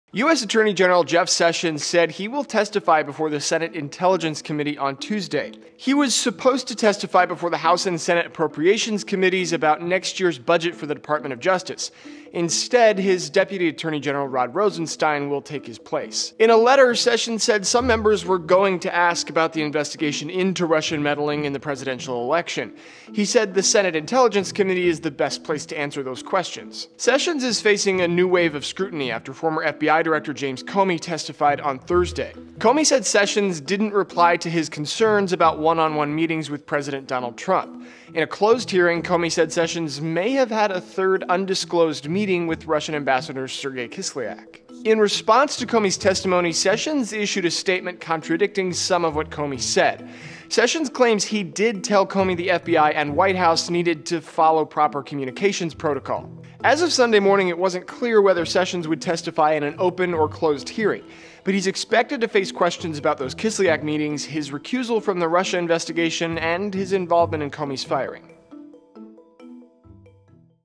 Sessions offers to answer questions about Russia
He wants a chance to respond to former FBI Director James Comey's testimony.